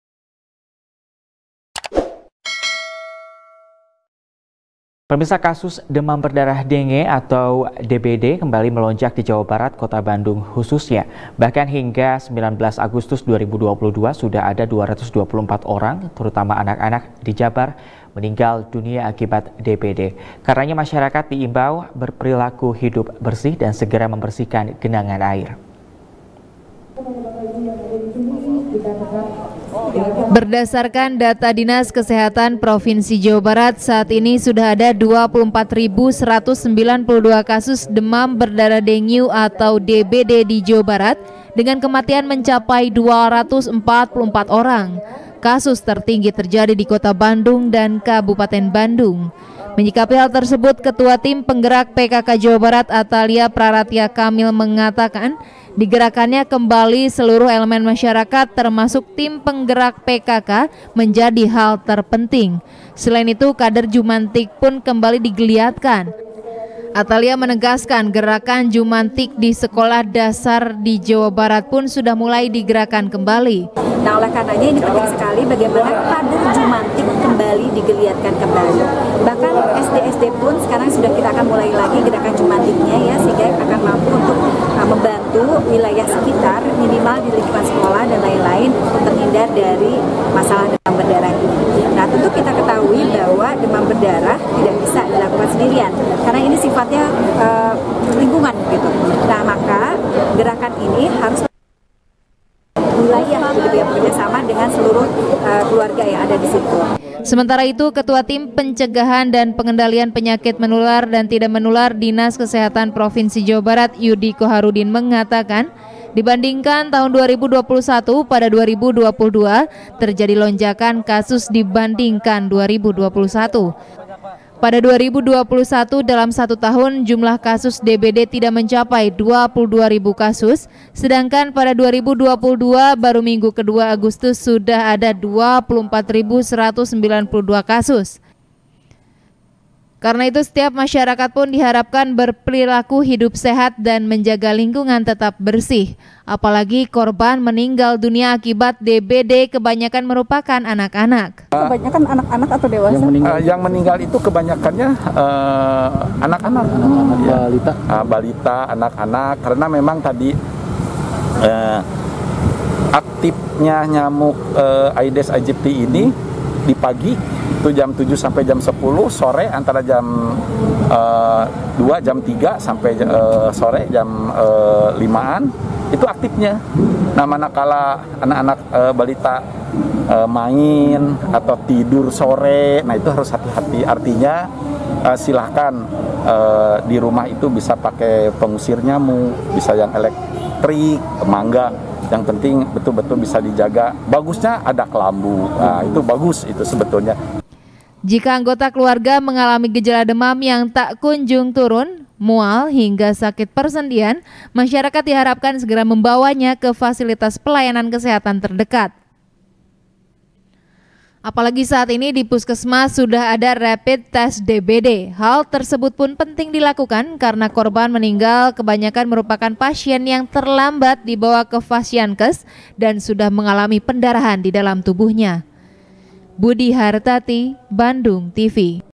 Kasus DBD Melonjak, Masyarakat Diimbau Berperilaku Hidup Bersih | SBR | BANDUNGTV
SBR | BANDUNGTV | IRFAN RAMANDA | Kasus Demam Berdarah Dengue Atau Dbd Kembali Melonjak Di Jawa Barat, Kota Bandung Khususnya. Bahkan, Hingga 19 Agustus 2022, Sudah Ada 224 Orang, Terutama AnakAnak Di Jabar, Meninggal Dunia Akibat DBD. Karenanya, Masyarakat Diimbau Berperilaku Hidup Bersih, Dan Segera Membersihkan Genangan Air,